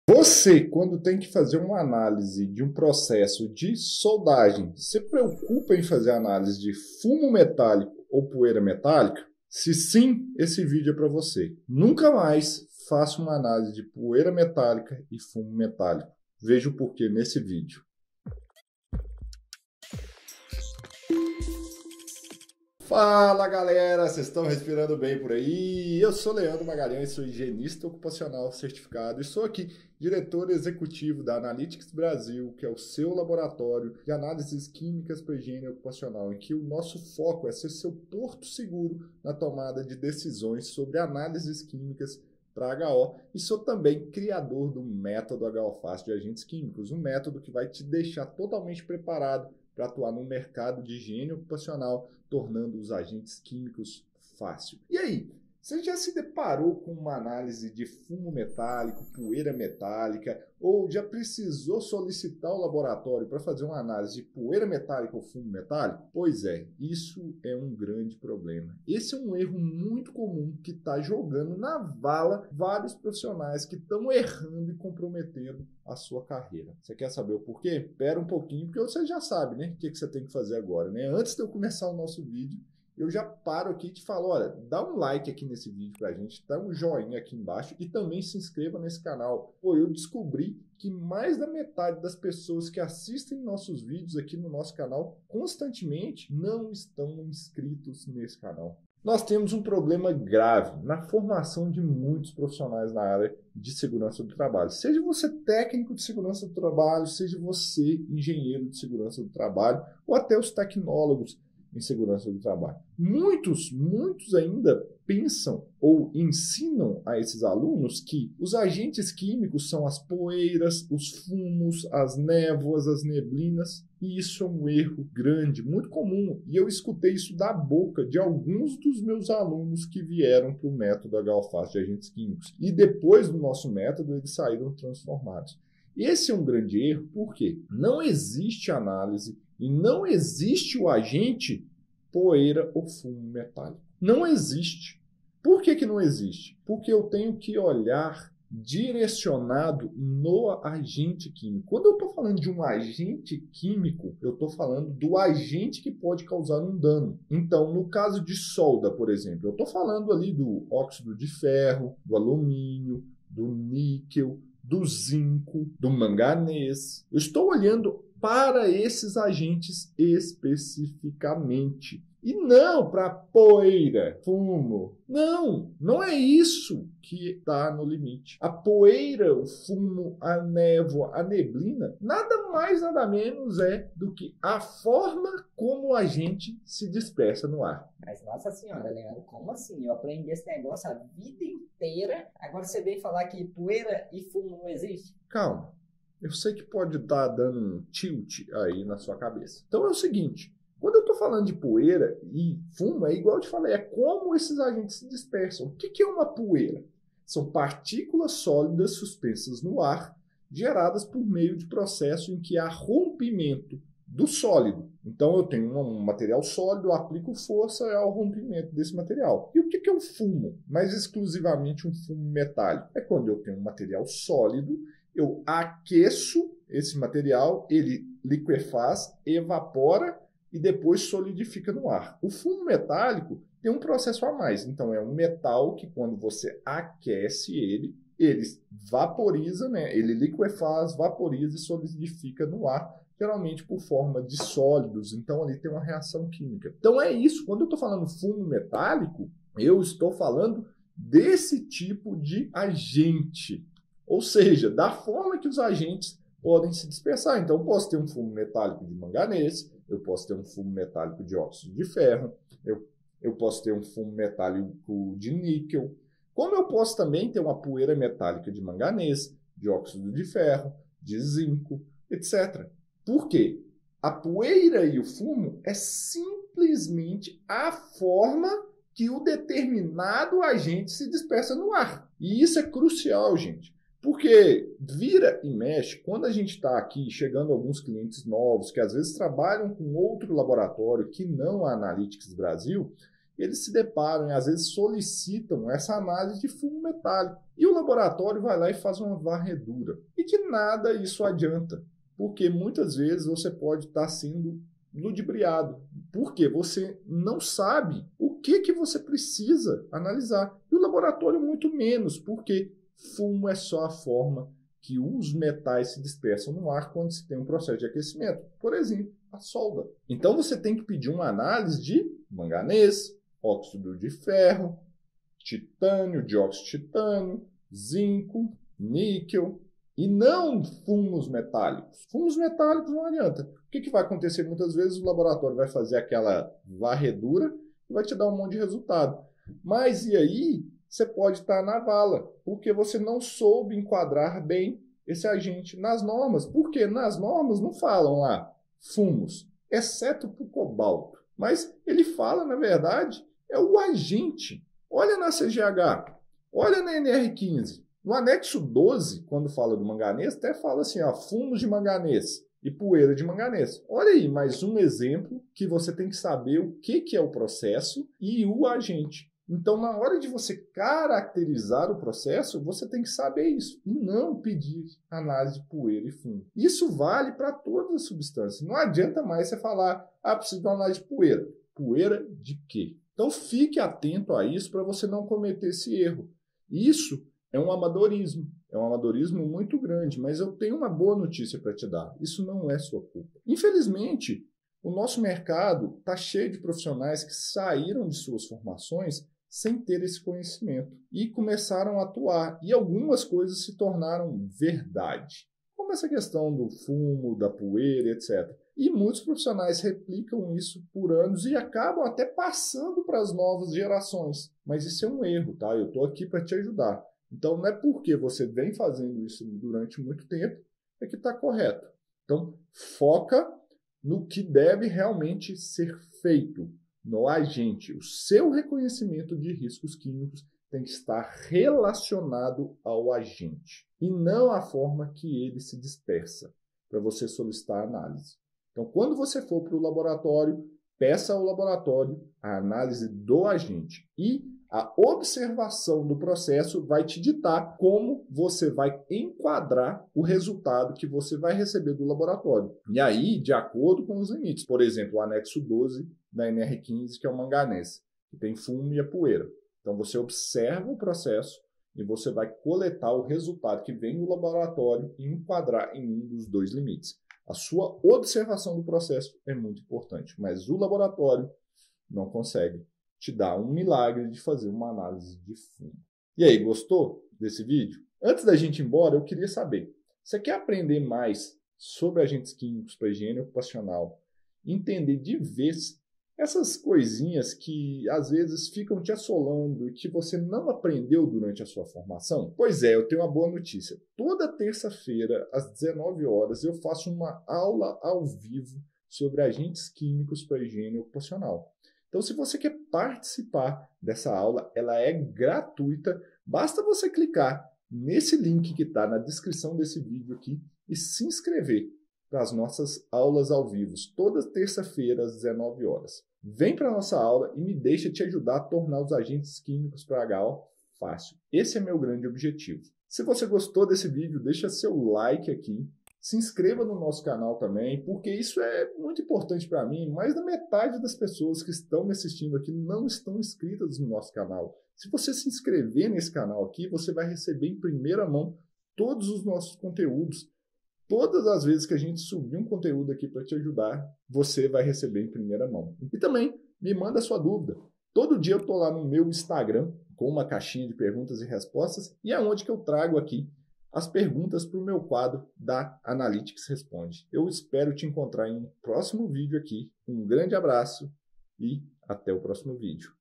Fumos Metálicos e Poeira Metálica nunca mais! - Higiene Ocupacional – AR #52
Muitos Profissionais as vezes ficam perdidos solicitando as análises erradas para situações de solda em que tem fumos metálicos. Veja neste vídeo como não errar mais na hora de solicitar essas análises.